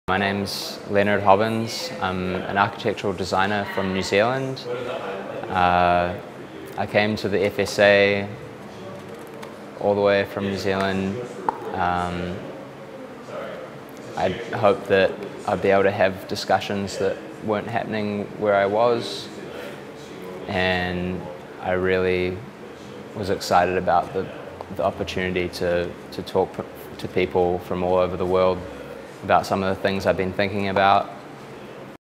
FSA Toolkit - Leonard
Free School of Architecture (FSA) '18 participant Leonard talks about his FSA experience.